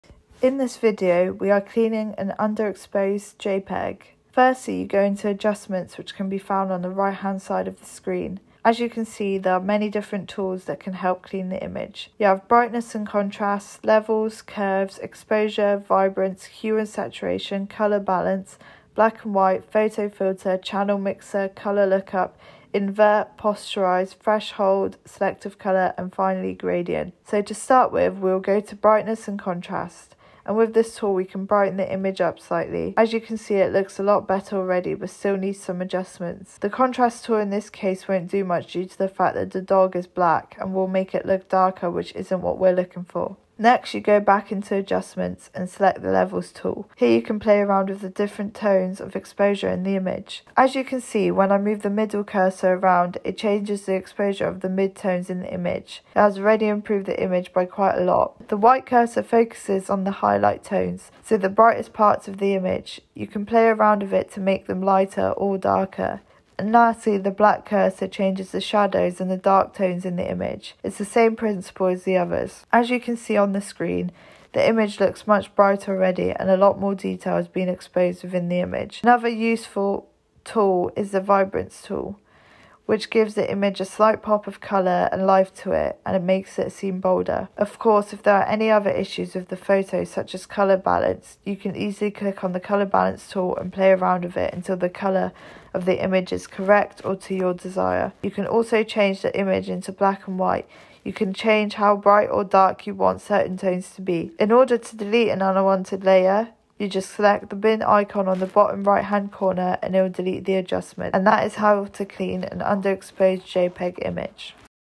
Basic guide to cleaning JPG images | Dominika Staroscik
Dominika talks through how to edit JPG images